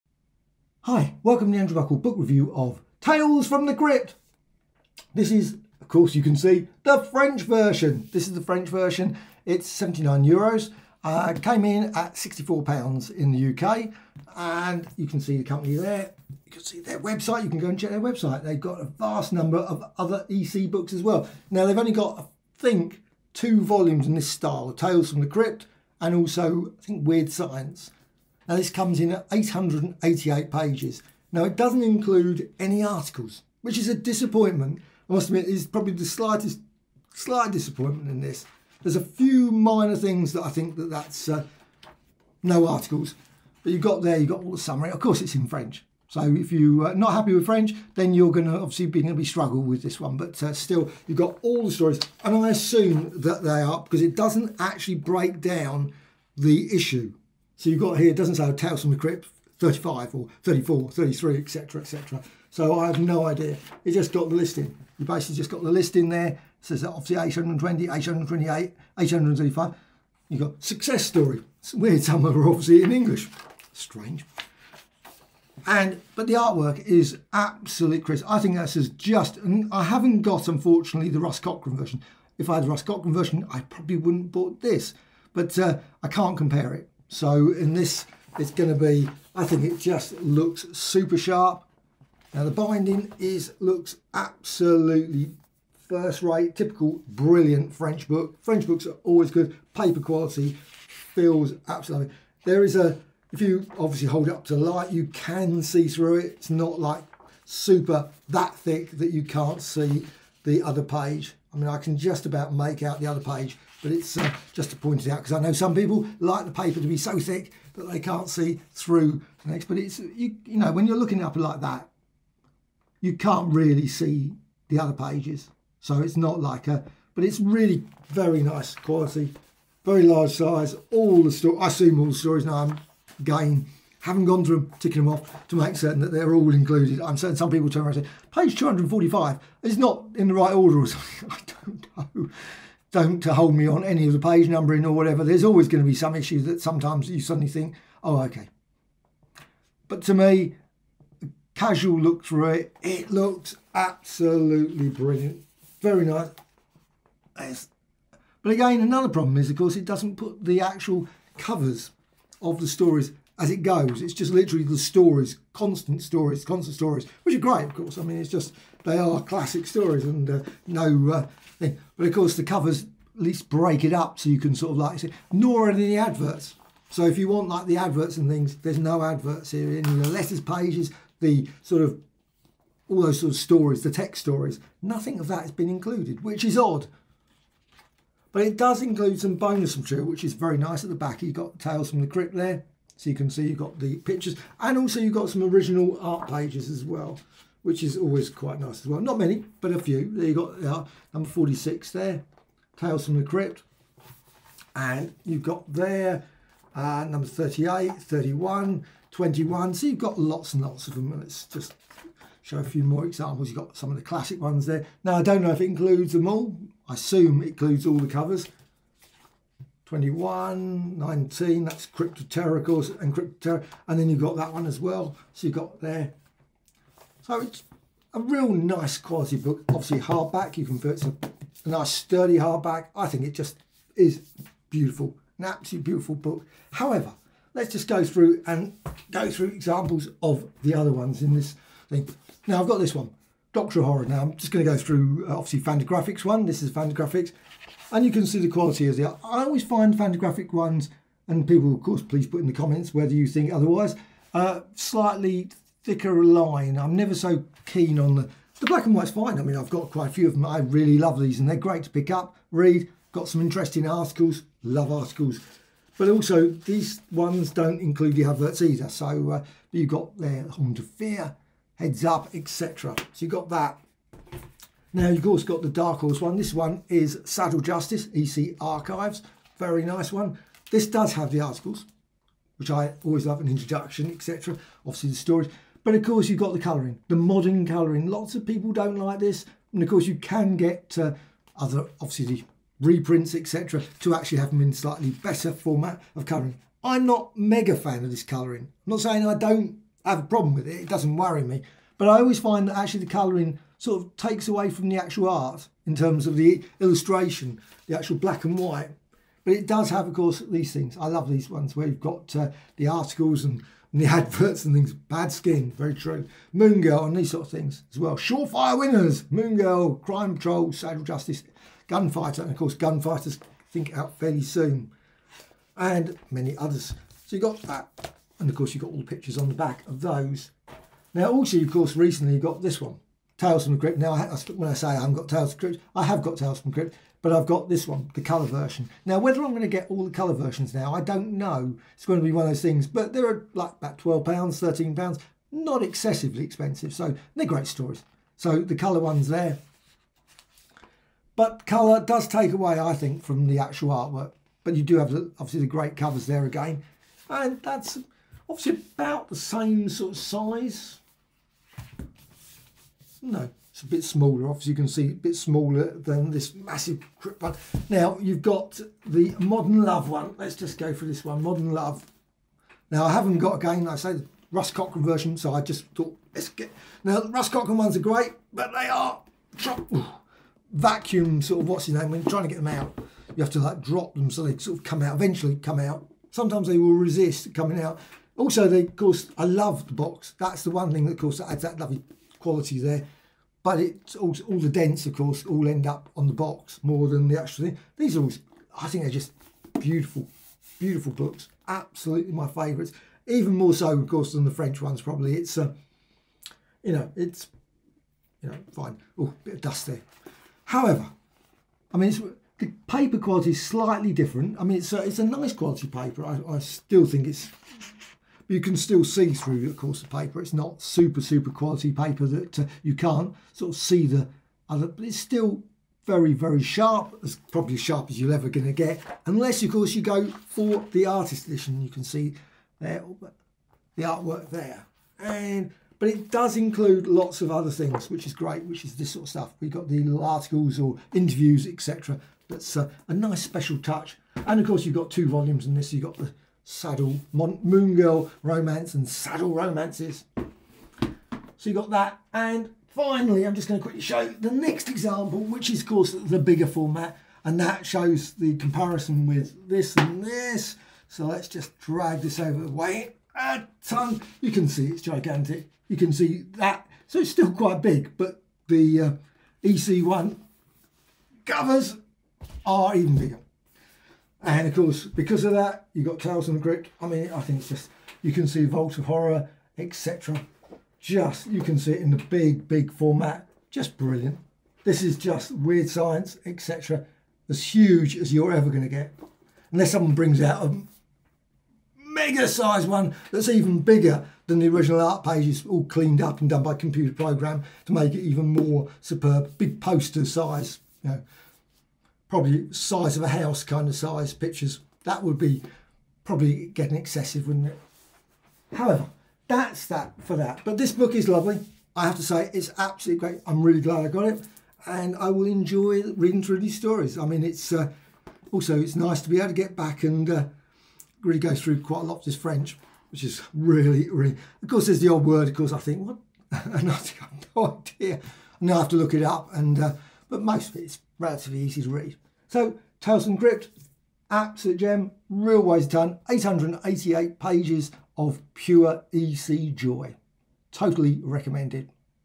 Tales From The Crypt EC Integrale French Akileos Book Review (Compare)
Tales From The Crypt EC Integrale French Akileos 1 Book Review (Compare with Artist edition, Dark Horse, Russ Cochran etc